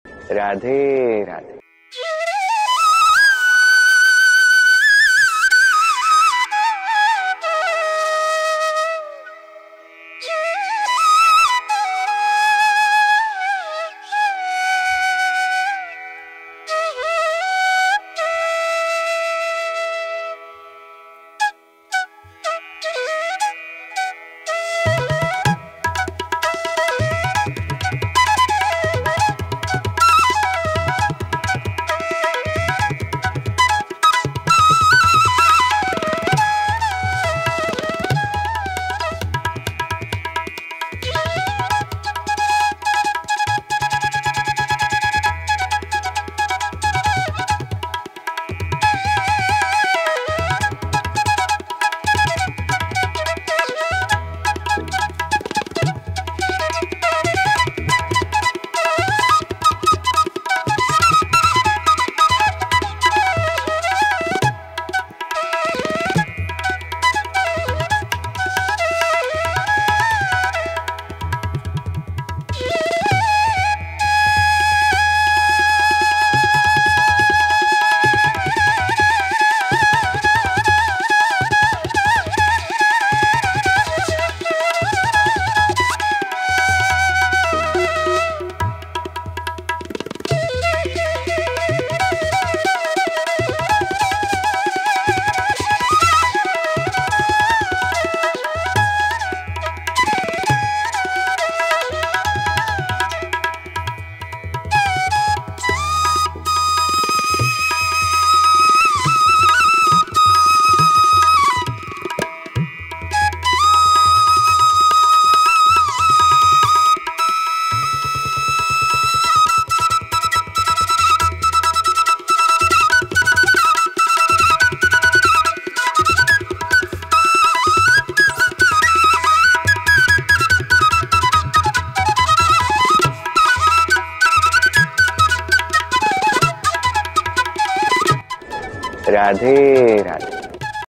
Radhe Radhe 🙏🌸🌟radhakrishna
This is a watercolour painting video, in this video I paint a very beautiful Radha Rani 🌸🙏 pls check it out, radhe radhe 🦚✨🪷❤️ This video is for educational and entertainment purpose only 😀 Thanks for reading 😌❣️
Meterial -
Sketch book - Doms a5 (150Gsm)
Colour- Camel Artist watercolour